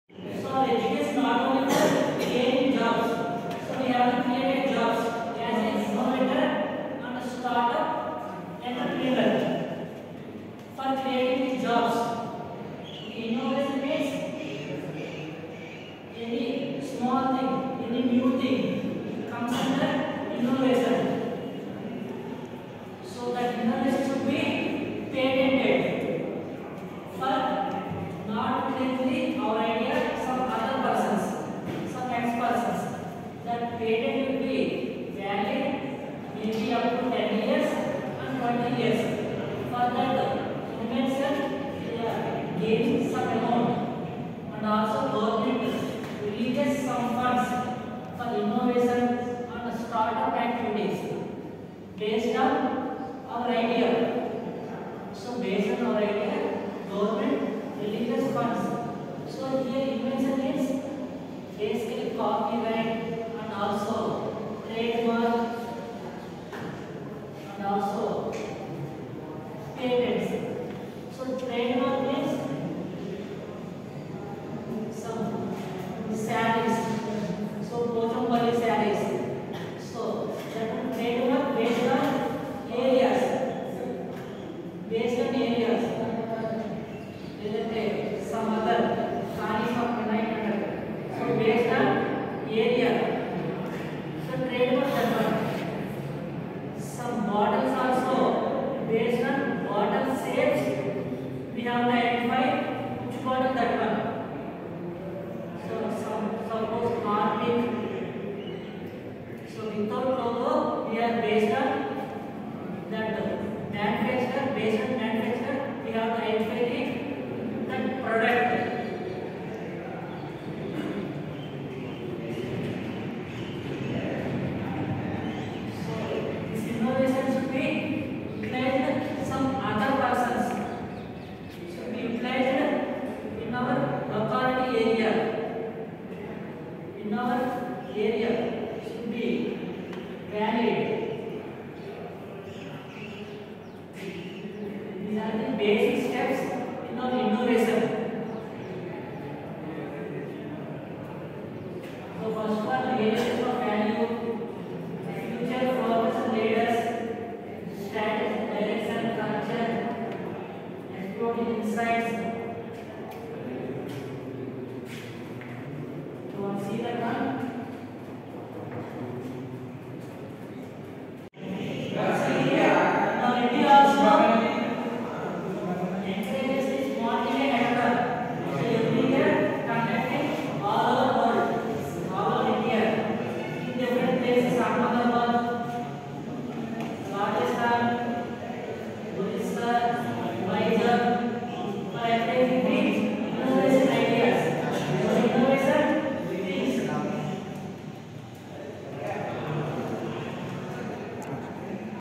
IIC Organizes Innovation & startup as a career opportunity on 2nd Feb 2023 as a self driven activity
IIC Organizes Innovation & startup as a career opportunity on 2nd feb 2023 as a self driven activity by Mr. B.Bharath Kumar, IIC MEMBER, Innovation Ambassador, ME Dept, Pragati Engineering College(A) for Mechanical second  year students.